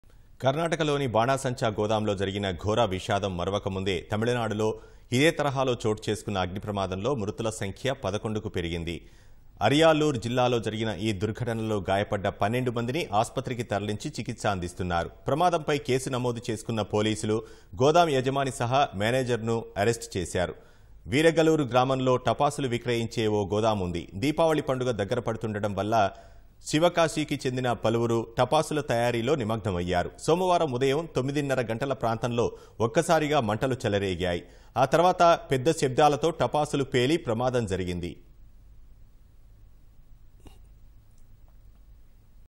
11 Dead In Fire At Tamil Nadu Firecracker Factory | తమిళనాడులో బాణాసంచా గోదాంలో అగ్నిప్రమాదం
కర్ణాటకలోని బాణాసంచా గోదాంలో జరిగిన ఘోర విషాదం మరువక ముందే తమిళనాడులో ఇదే తరహాలో చోటుచేసుకున్న అగ్నిప్రమాదంలో మృతుల సంఖ్య 11కు పెరిగింది. అరియాలూర్ జిల్లాలో జరిగిన ఈ దుర్ఘటనలో గాయపడ్డ 12 మందిని ఆస్పత్రికి తరలించి చికిత్స అందిస్తున్నారు. ప్రమాదంపై కేసు నమోదు చేసుకున్న పోలీసులు. గోదాం యజమాని సహా మేనేజర్ ను అరెస్టు చేశారు. వీరగలూరు గ్రామంలో టపాసులు విక్రయించే ఓ గోదాం ఉంది. దీపావళి పండుగ దగ్గర పడుతుండడం వల్ల శివకాశీకి చెందిన పలువురు.. టపాసుల తయారీలో నిమగ్నమయ్యారు. సోమవారం ఉదయం తొమ్మిదిన్నర గంటల ప్రాంతంలో ఒక్కసారిగా మంటలు చెలరేగాయి. ఆ తర్వాత పెద్ద శబ్దాలతో టపాసులు పేలి ప్రమాదం జరిగింది.